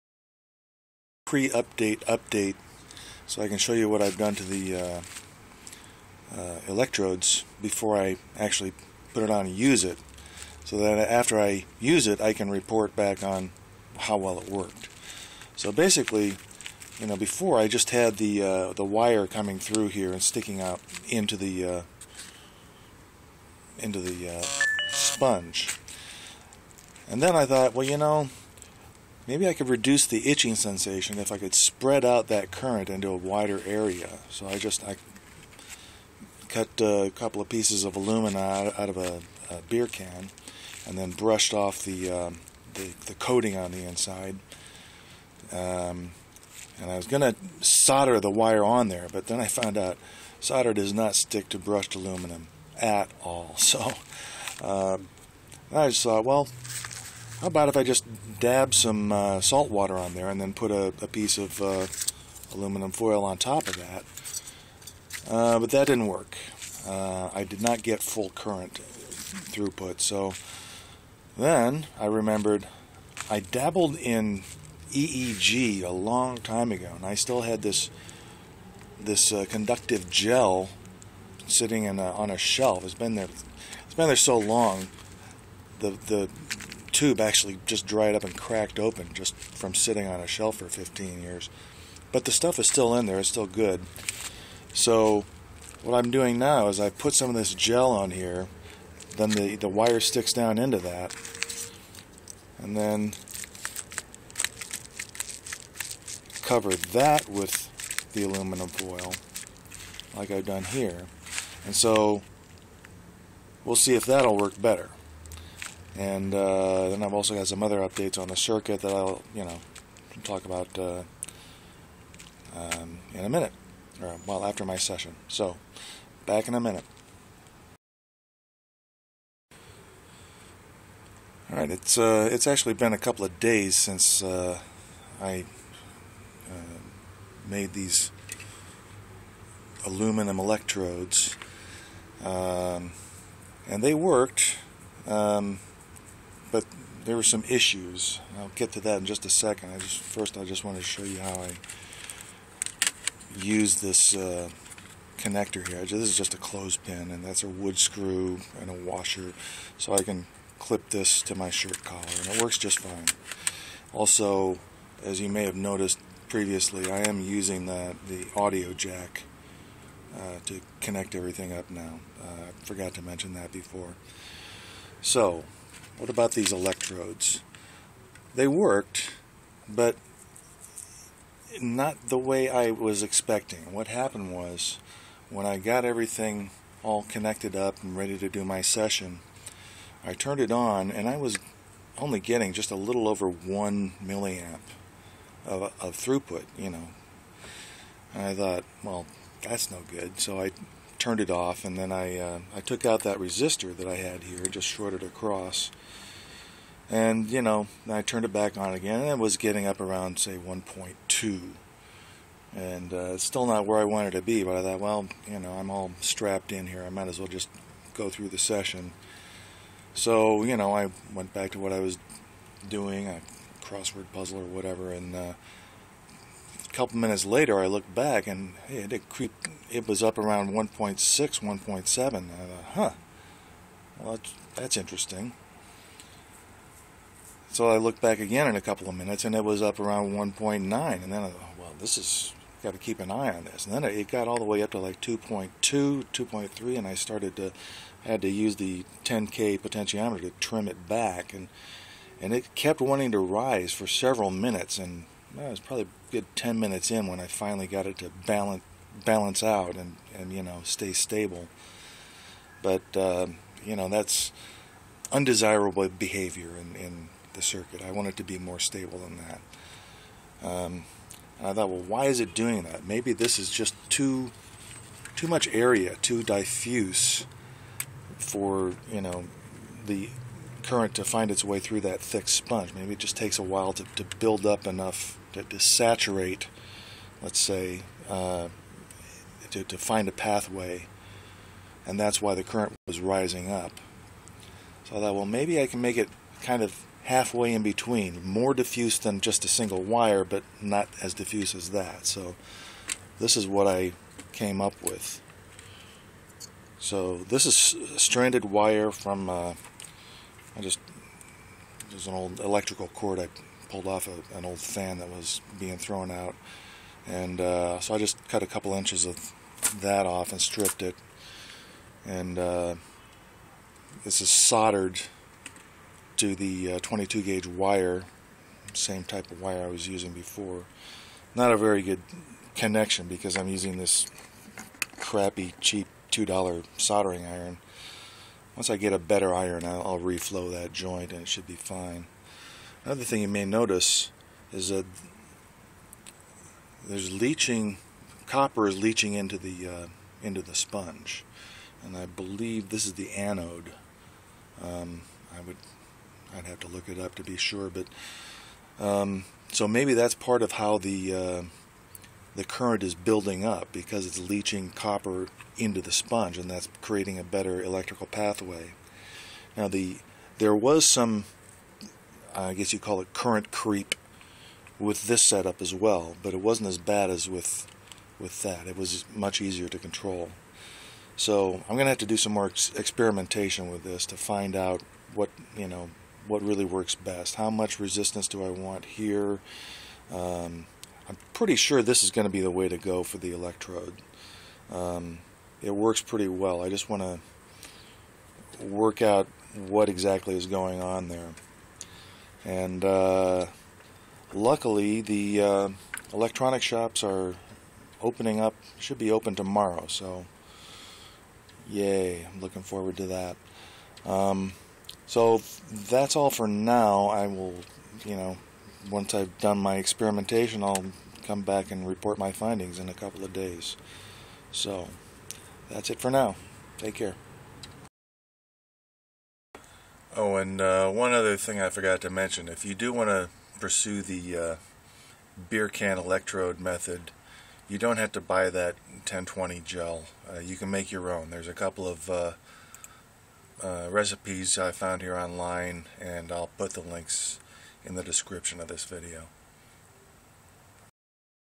My tDCS Experiment: Part 5: Optimizing the Electrodes (sorta...)
I decided to explore some better ways to connect the wires to the sponges to get a more even distribution through the electrode, and came up with some interesting results. I think I know what's going on, but will need to do some more experiments to be sure.

In the meantime, here are the links mentioned above, for anyone who might want to create their own DIY conductive gel...